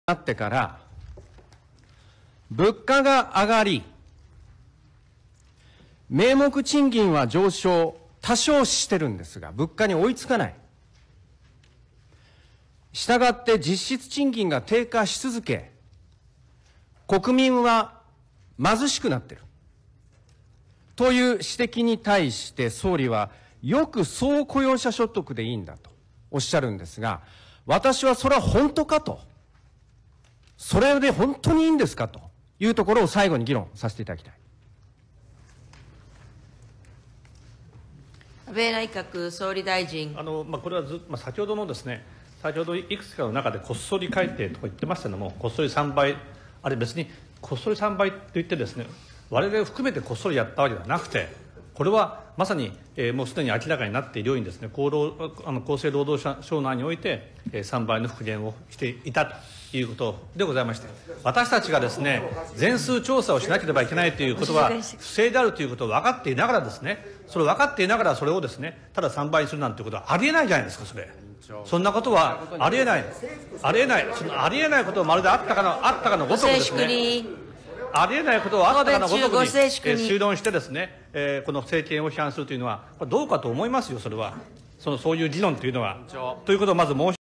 【安倍の得意】そんなこと、あり得ないじゃないですか ？？何で？？？？？？？scratch 小学生 政治に立ち上がる
引用：19/2/18　衆院予算委員会

何であり得ないのかは、何も述べていない。やってることは声を大きく挙げるだけ。